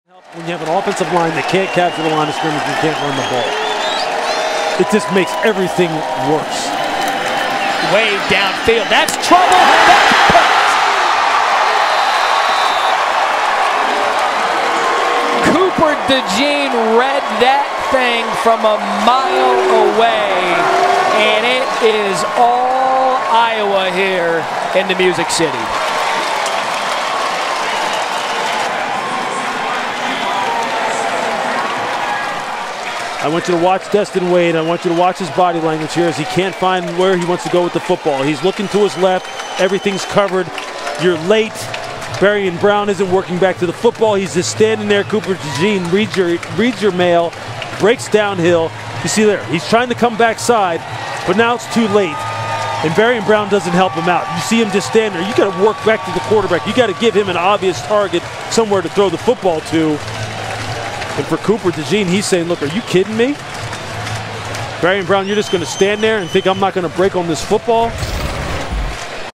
Iowa scores on ANOTHER pick six vs Kentucky | 2022 College Football
During the college football game between Iowa and Kentucky, Iowa scored their second pick six of the game to take a 21-0 lead in the second quarter.